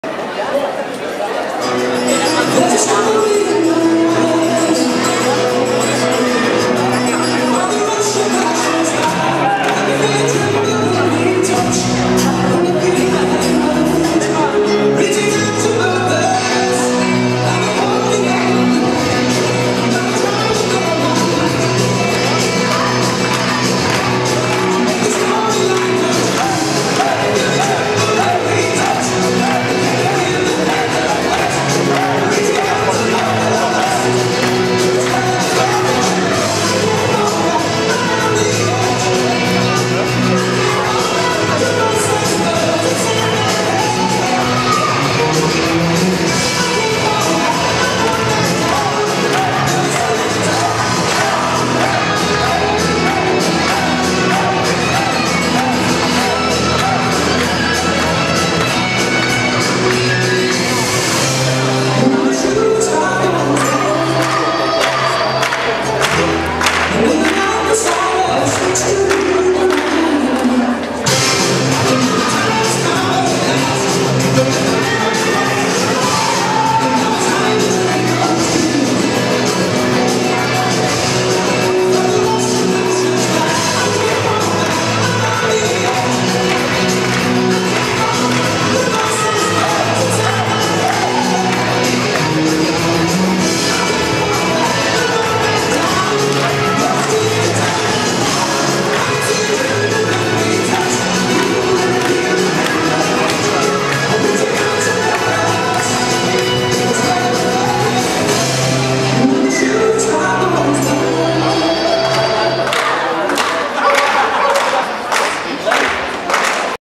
Louisa & Benjamin Rock 'n' Roll B-Klasse Akro Kreuztal 2009
Norddeutsche Meisterschaft, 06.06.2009, Teddybears Iserlohn, B-Klasse, Akrobatik, Finale, Platz 2